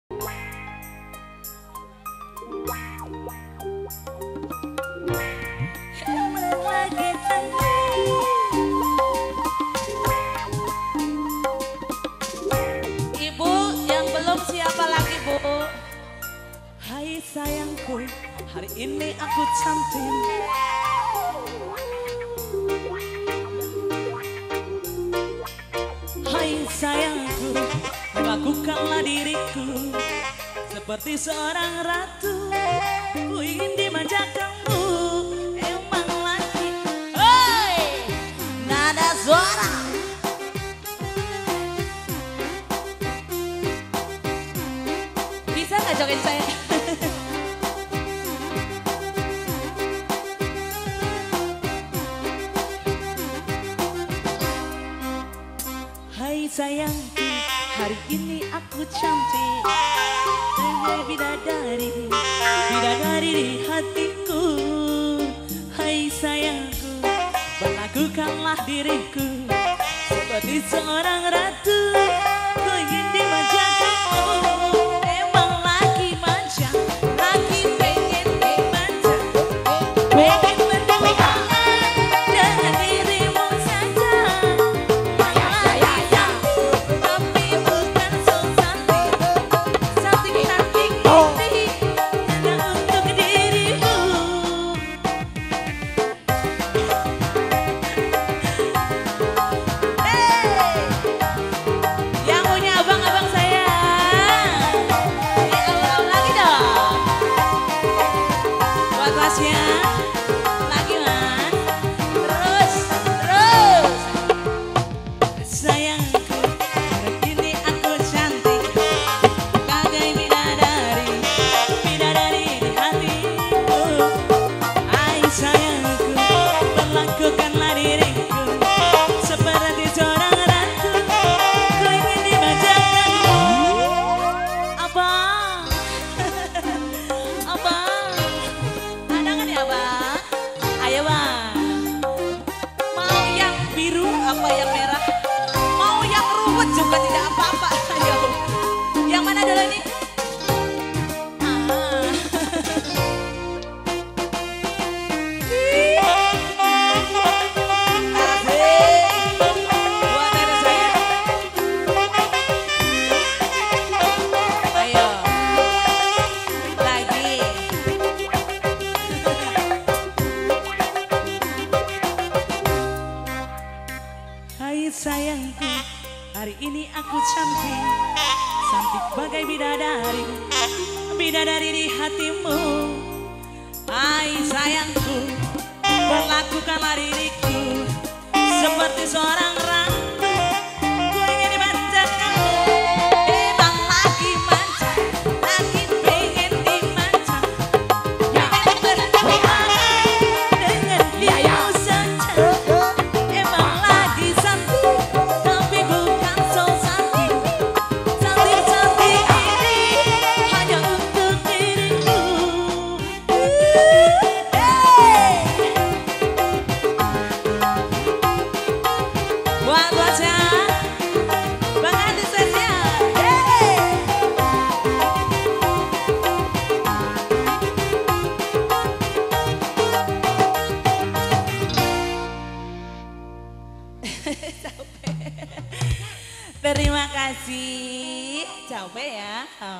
13 NADA SWARA ENTERTAINMENT @ Ranjiwetan Kasokandel Majalengka
Adi Karya Ranjiwetan, 17 Februari 2019
Pernikahan Enceng Didin A.J & Ayu Sadiah
Enceng & Ayu

Keluarga Hajat
Bpk. Toha - Ibu Icih Arsih
Kel.Bpk. Salim (Alm) - Ibu Uum Umirah
Kel.Bpk. Sadi (Alm) - Ibu Sutijah (Almh)

Blok Kamis Ranji RT 01 RW 07 Ranjiwetan Kasokandel Majalengka

Akad Nikah 18 Februari 2019

NADA SWARA ENTERTAINMENT Pimp. Abah Ikin Tarikolot Desa Ranji Wetan VIDEO DOKUMENTATION BY